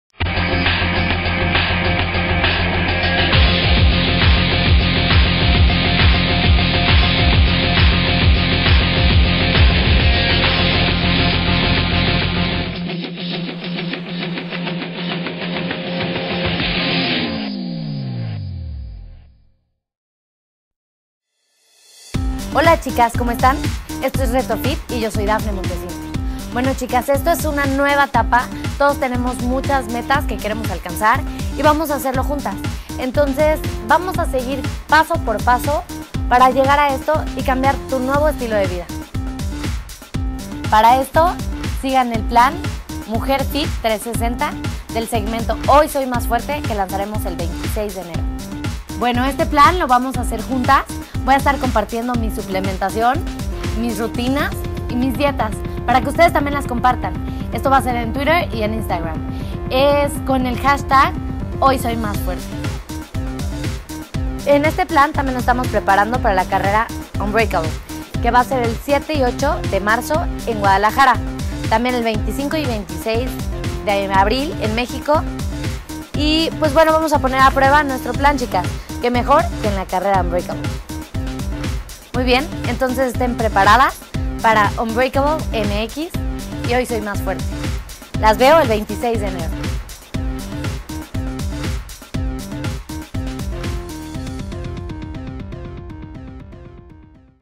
Hoy Soy Más Fuerte / Prepárate para Unbreakable Proximamente
Hermanos estamos muy cerca de lanzar los planes que van a transformar su estilo de vida por completo. Estos planes los haremos con ustedes y estaremos compartiendo cada paso en FB y TWT con el HoySoyMásFuerte así que únanse y entrenen con nosotros. Nos vemos el 26 de enero.

Reto Fit es tu canal de entrenamiento, descubre rutinas, encuentra ejercicios, motívate y no faltes a tus entrenamiento. Todo esto se te recompensara aquí, en Reto Fit.